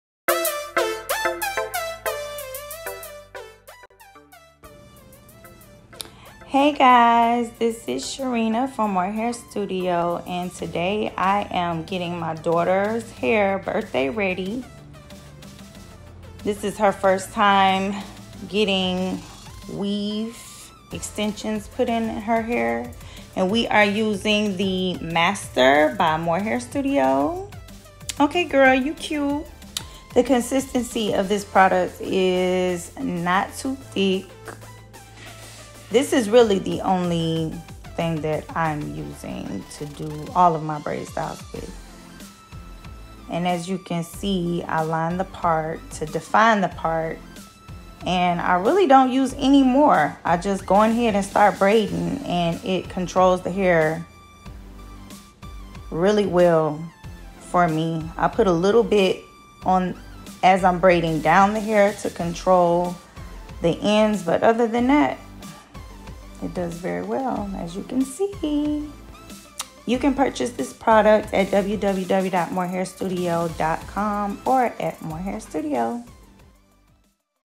The Master Braid Gel
- You don’t need a lot.
- Controls the hair without making it hard
- Lasting Results

Use with your favorite mousse & hair oil & voilà…perfection! ✨

Official product review coming soon……..

$12.00 each
2/$20.00 code: mastermhs